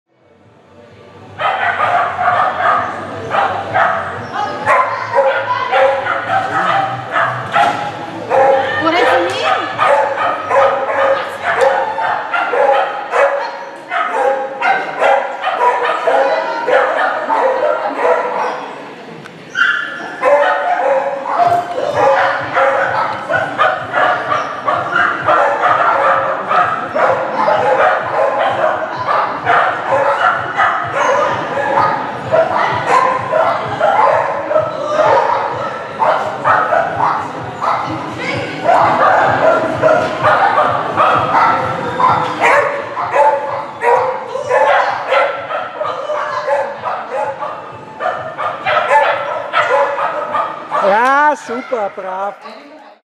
1 VBSÖ Cup Haunis DSC Agility Team Martina mit Ulani beim A Lauf 10.11.2013
Das Hauni Agility Team beim Prüfungslauf in allen Leistungs- und Größenklassen  A-Open, J-Open

Das Team mit: Sabine Beyer mit Buffy vom wilden Hexenkessel, Vanessa Beyer mit Beagle Amy und Bonny von der Haunimühle,Walter Bodenstab mit Jimmy, Martina Wolf mit Ulani, Jasmin Bodenstab mit Esprit